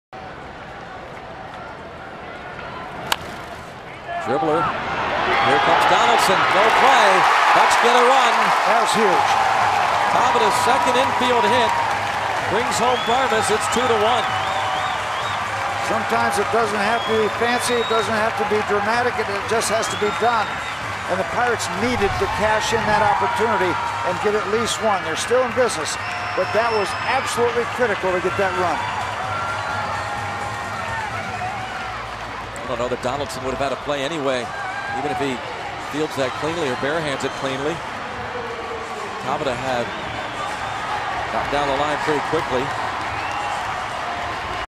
OAK@PIT: Tabata taps infield grounder to score a run
7/8/13: Jose Tabata hits a soft ground ball that stays fair for an RBI single, cutting the A's lead to 2-1 in the seventh inning